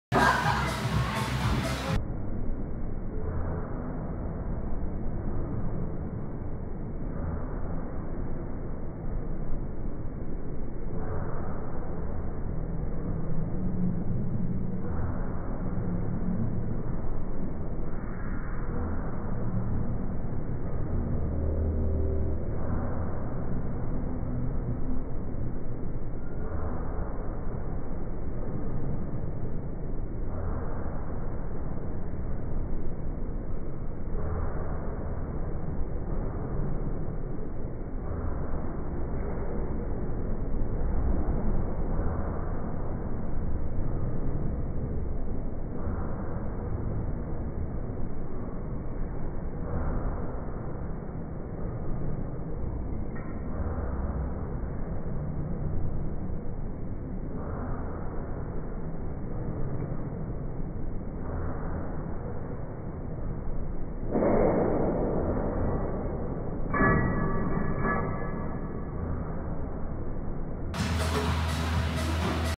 SAFETY MAXX OLYMPIC BENCH by BODYTASTIC #4
Kelly in SLO MO this time- just because we can and it looks awesome!